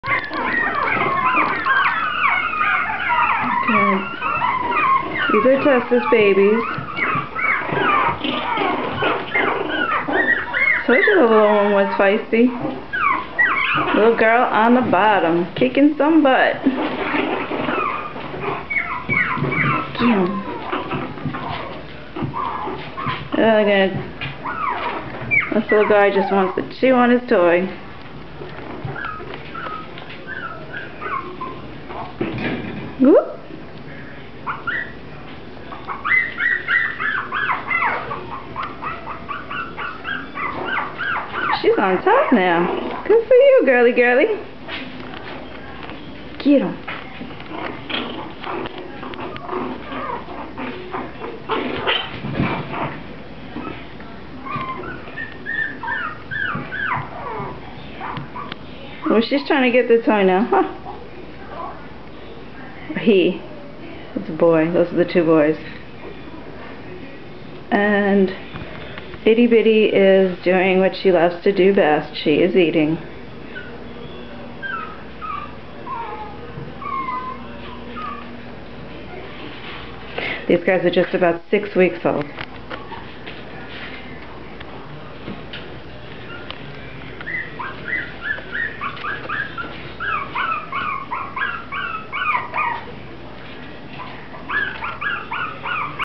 Tiny Treasure Mi-Kis Tessa and Knoxx's puppies...6 weeks old...
Tessa's babies are getting bigger...playing, eating and learning their potty training skills :)